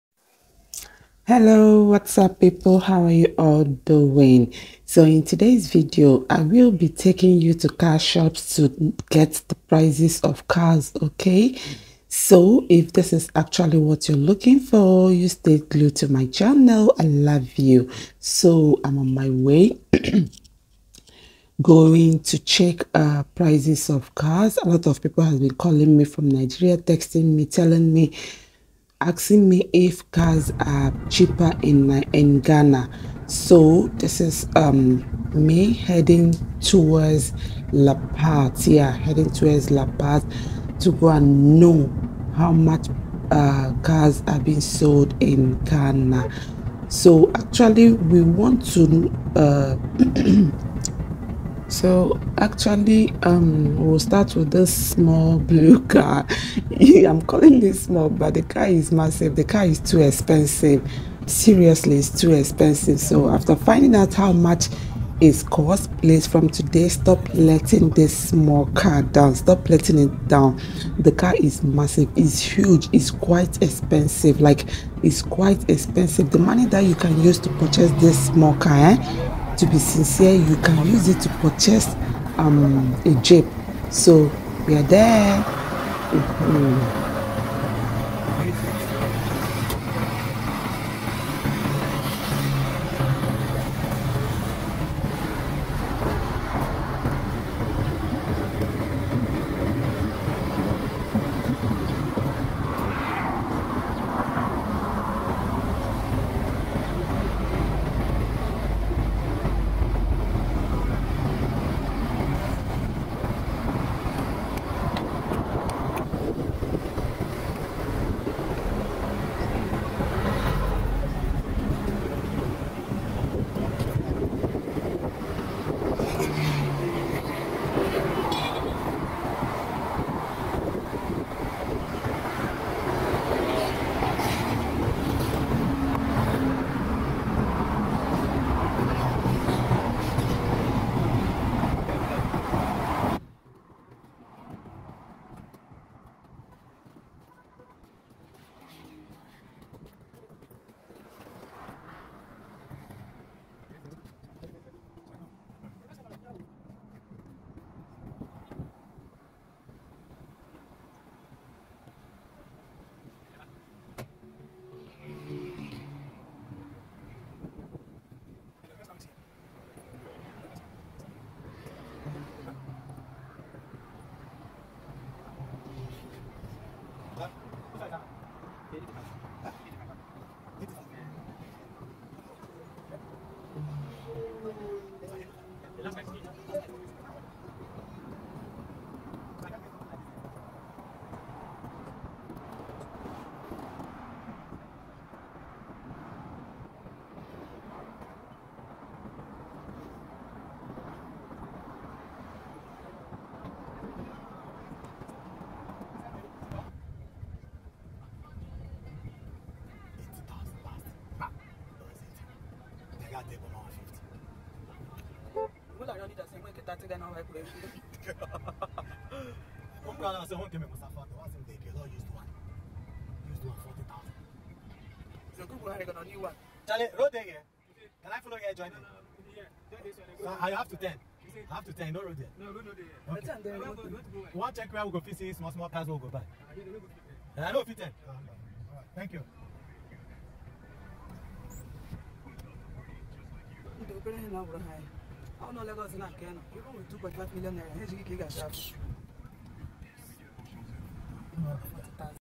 PRICES OF GHANAIAN USED CARS THAT WILL BLOW YOUR MIND.
Get ready to be shocked by the unbelievable prices of Ghanaian used cars in this eye-opening video! You won't believe the amazing deals and steals you can find on these vehicles that will make your jaw drop. the price tags on these gems will blow your mind. Join me on a journey through the Ghanaian car market and prepare to be amazed at the incredible bargains waiting for you. If you're in the market for a new set of wheels or simply love a good deal, this video is a must-see!